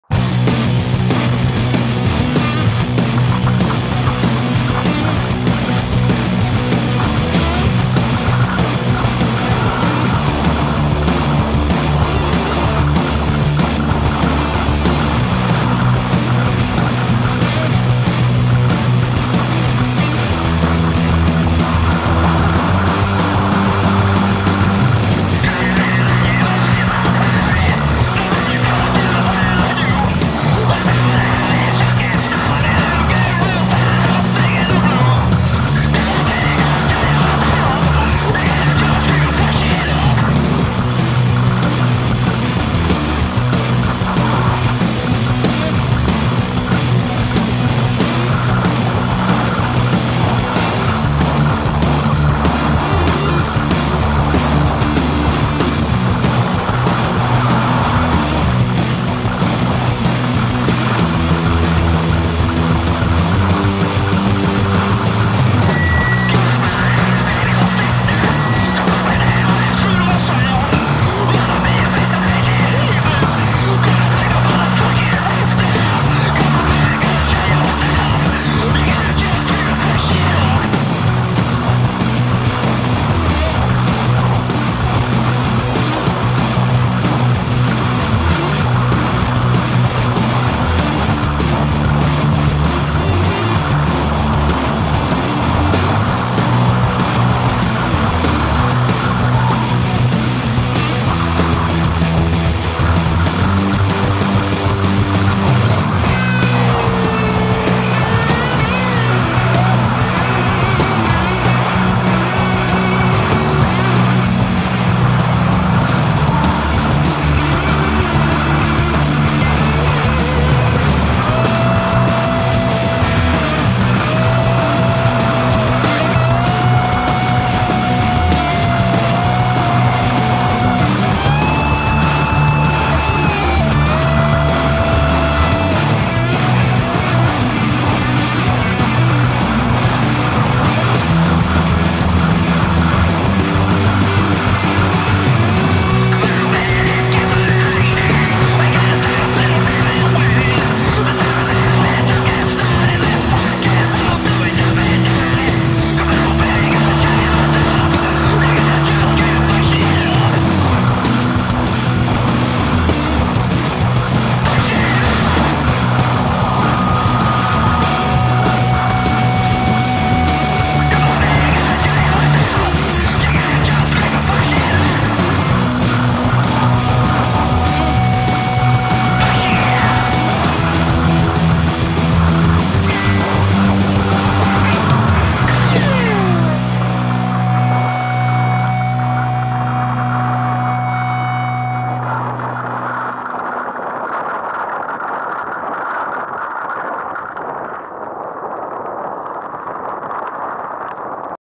Cannock Chase Monkey Trail - Upper Cliff Descent
Seatpost mounted rear facing camera - Sept 2014